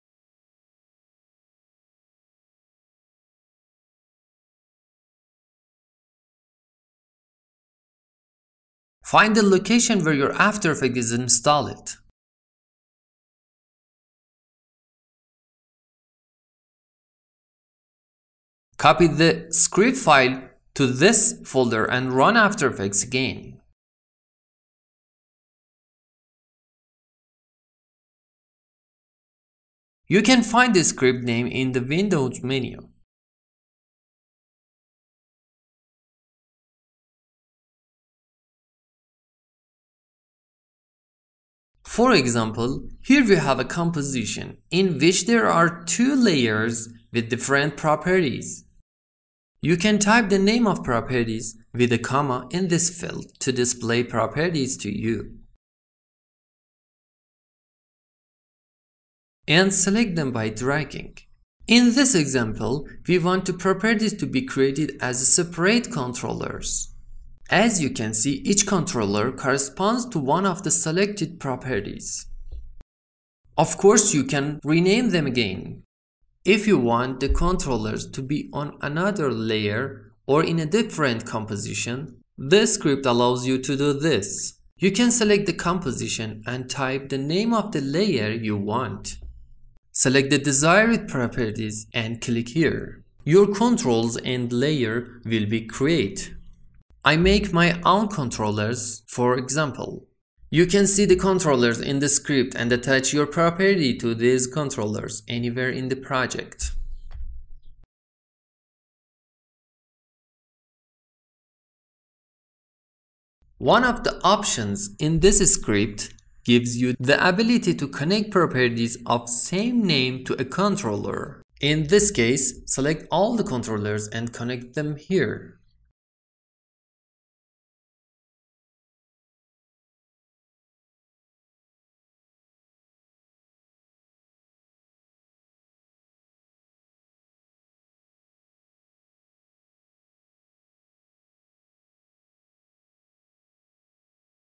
Add Expression Controllers AE Script
After Effects users need to build Expression Control.
Sometimes the problem is much more complicated than this process.
In many nested properties, select too many layers, attach them to a particular layer, Or a special composition.

If you need a script to create Expression Control of your selected property in adobe after effects ,see how this script will do it & download it.